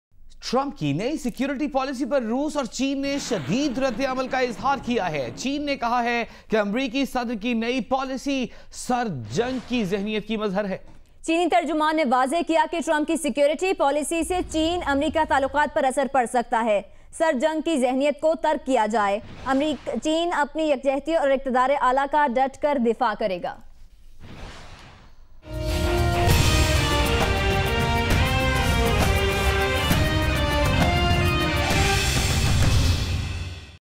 China and Russia denounced a US report on national security strategy
Special Report - China and Russia denounced a US report on national security strategy